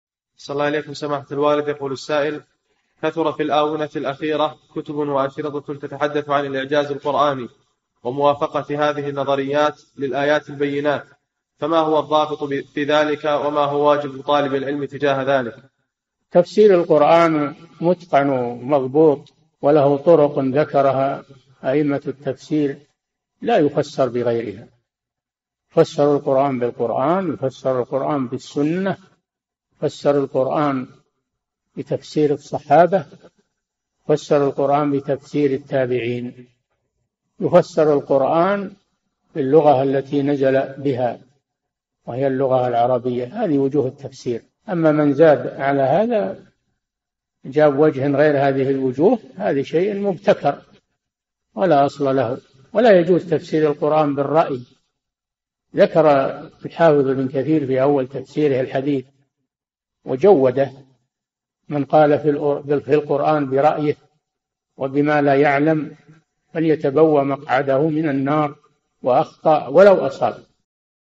[86] طرق تفسير القرآن الكريم - الشيخ صالح الفوزان
"[4717-86] كثر في الآونة الأخيرة كتبٌ وأسئلة تتحدث عن الإعجاز القرآني وموافقة هذه النظريات للآيات البينات، فما هو الضابط في ذلك وما هو واجب طالب العلم تجاه ذلك؟
الجَوَابُ: تفسير القرآن متقن ومضبوط وله طرقٌ ذكرها أئمة التفسير لا يُفسَّر بغيرها، يُفسر القرآن بالقرآن يفسر القرآن بالسُّنَّة، يفسر القرآن بتفسير الصحابة، يفسر القرآن بتفسير التابعين، يفسر القرآن باللغة التي نزل بها، وهي اللغة العربية هذه وجوه التفسير، أما من زاد على هذا وجاء بوجهٍ غير هذه الوجوه هذا شيءٍ مبتكر، ولا أصل له، ولا يجوز تفسير القرآن بالرأي، ذكر الحافظ ابن كثير في أول تفسيره الحديث وجوده «من قال في القرآن برأيه وبما لا يعلم فليتبوأ مقعده من النار وأخطأ ولو أصاب».

للاشتراك في قناة الشيخ صالح الفوزان
فتاوى المجالس في تفسير المفصل

للاستفسار والإقتراحات والدعم
رئيس الفريق
لمشروع كبار العلماء